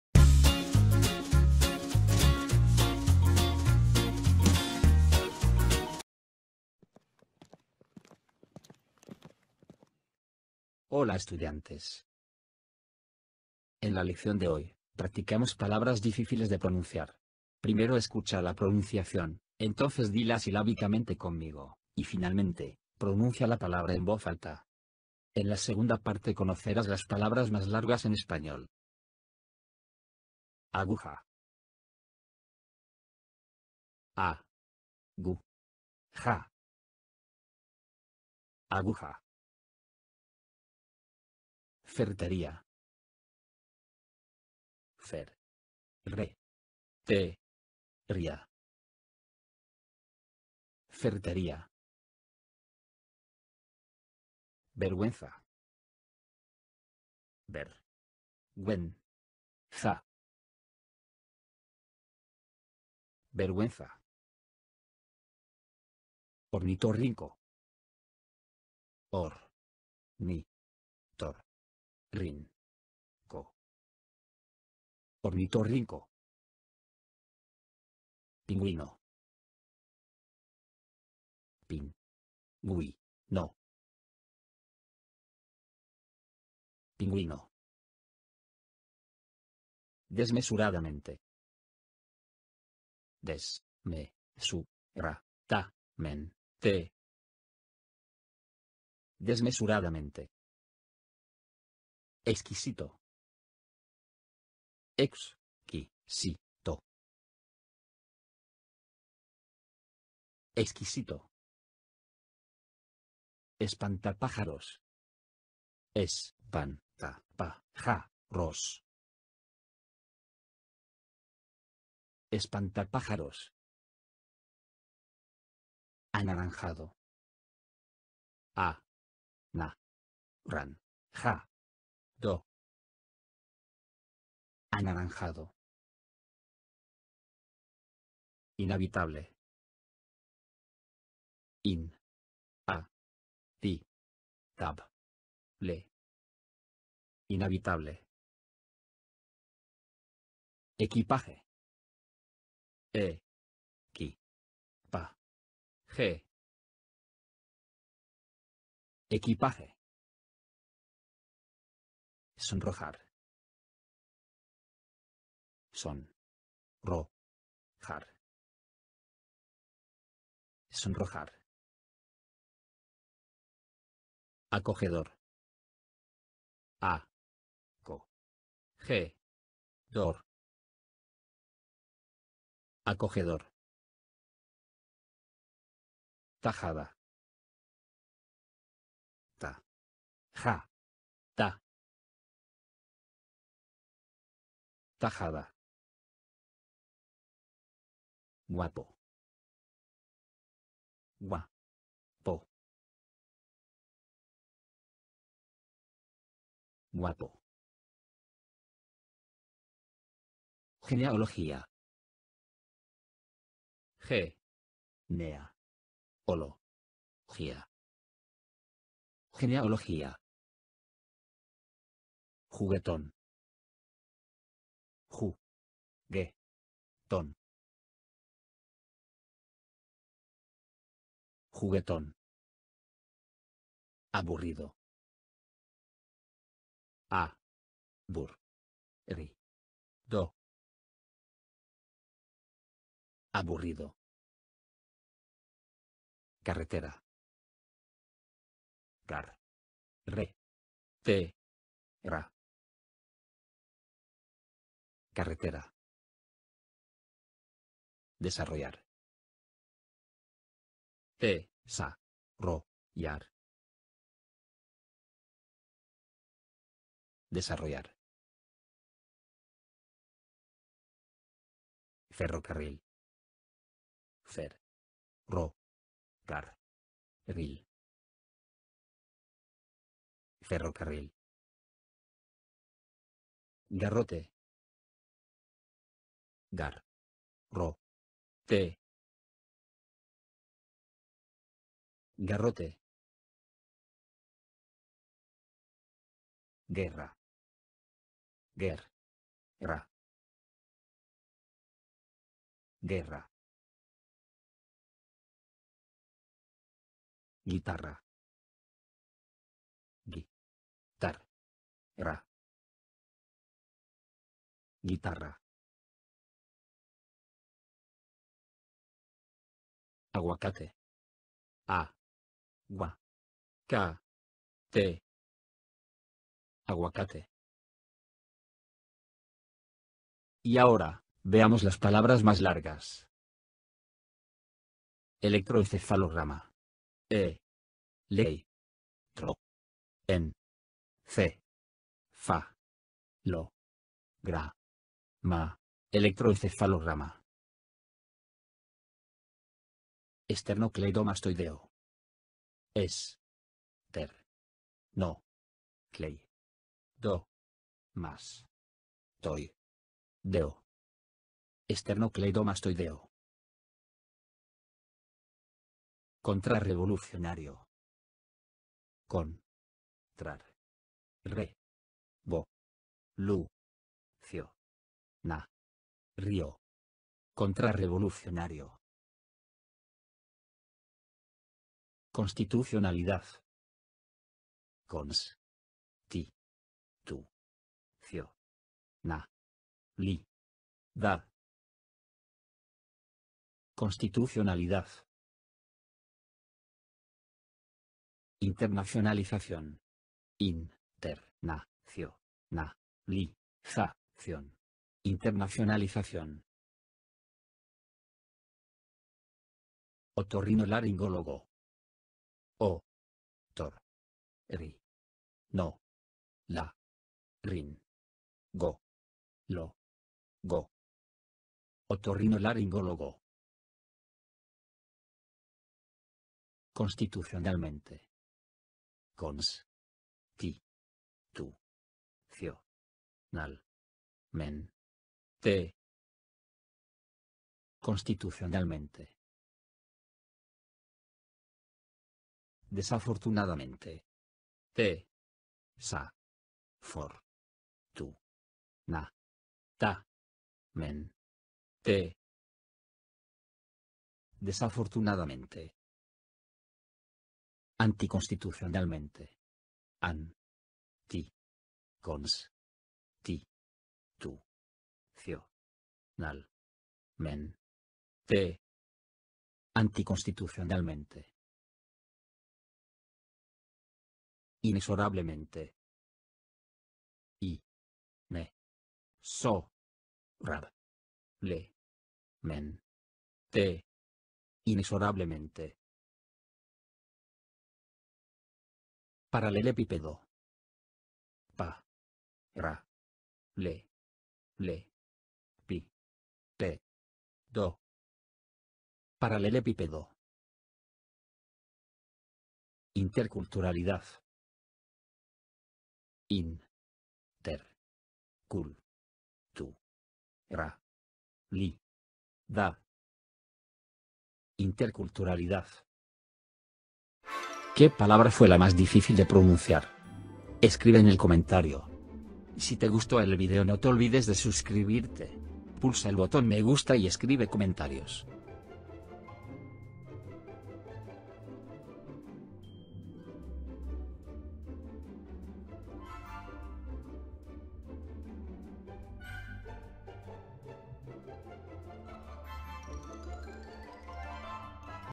Spanish Hard To Pronounce Words| Spanish Vocabulary
In today's lesson we practice hard-to-pronounce words.In the first part of the video you can learn words that are difficult to pronounce then in the second part you will know the longest words in Spanish.